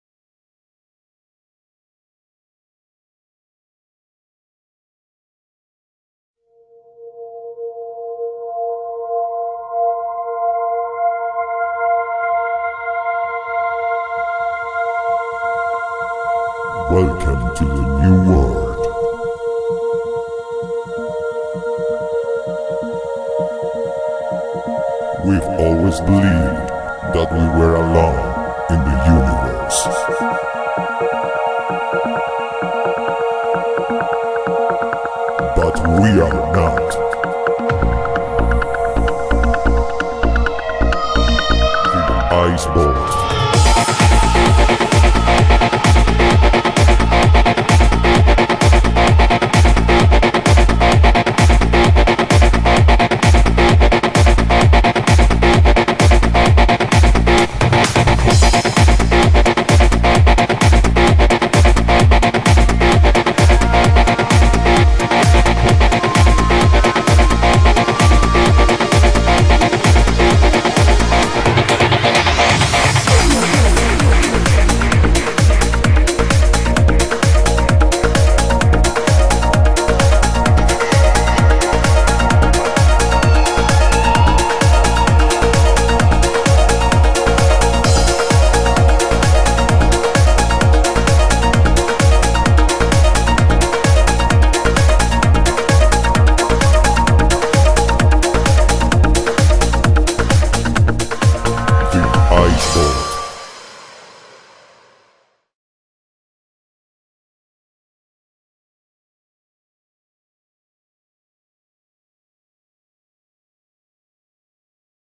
Ice-Bot Alive Robot - Contratacion Directa - "Rapport producciones"
Robot de 3 mts. de altura con 60 mts de Leds RGB, con 2 rayos laser y 1 disparo de Fuegos en cada Brazo, por ultimo Co2 GUN con 1 tanque de 25 kg para rociar con gas frio (CO2) a los invitados dejandolos helados, si lo que queres es sorprender a todos, es una novedad, 1 caja simulada de 3 mts de altura con ruedas para el ingreso del robot en la presentación (Caida de tela para la aparición del Ice-Bot). 2 Asistentes Lookeados. DURACION 35 MINUTOS